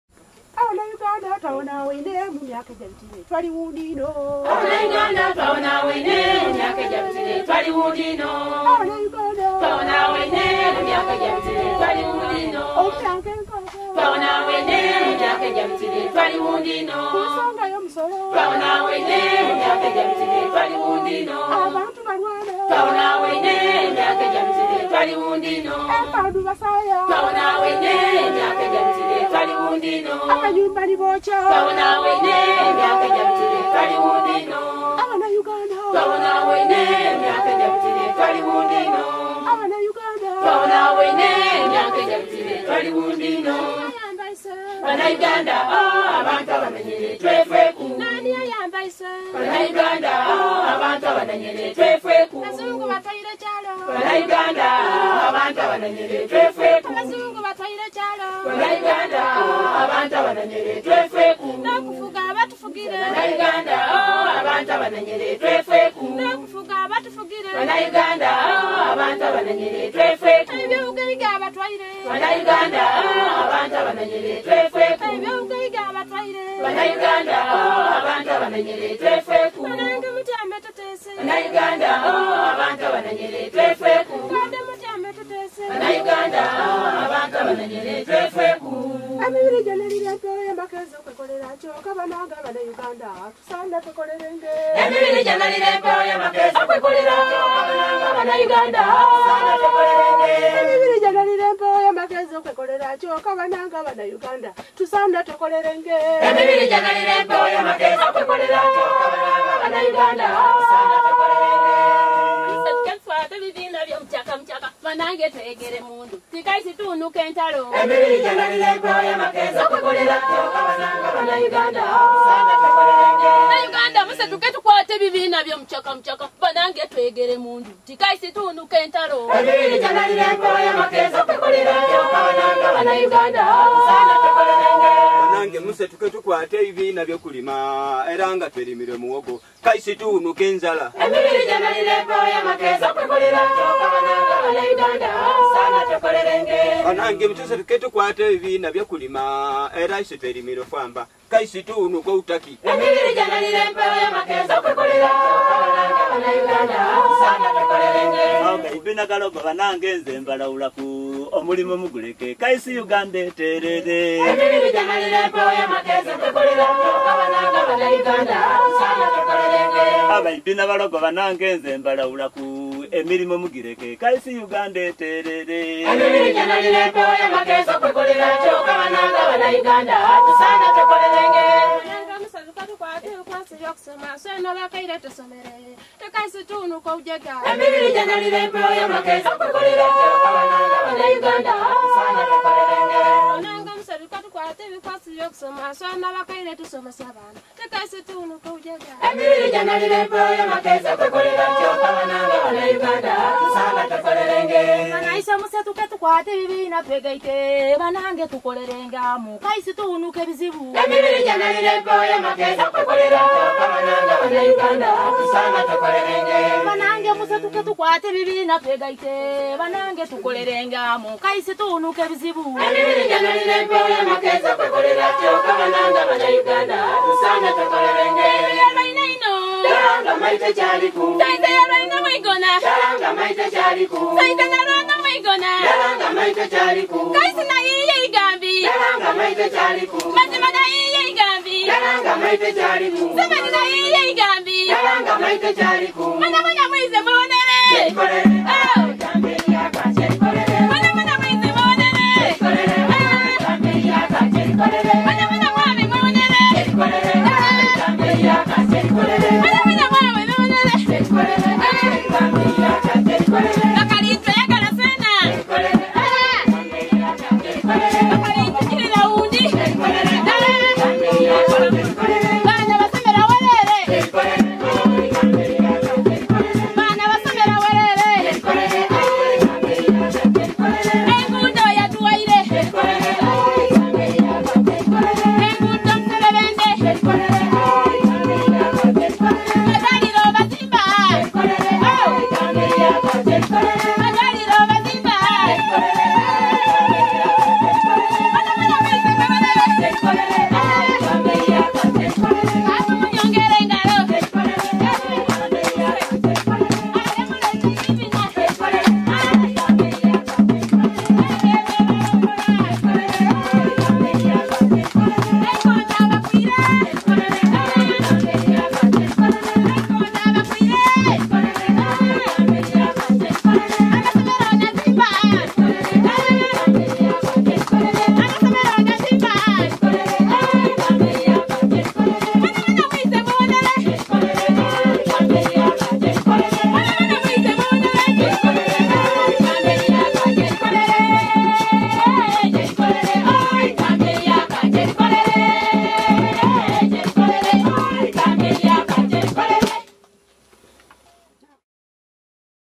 Eastern Uganda - choir and drums
Onswamoiza Budaka County Women's Group. In 2003 Teo Joling made a journey through Uganda. In East Uganda Pius Ochan introduced him to traditional musicians and dance groups - some led by his own ex-students. Godfrey Isabirye offered to drive them to remote locations.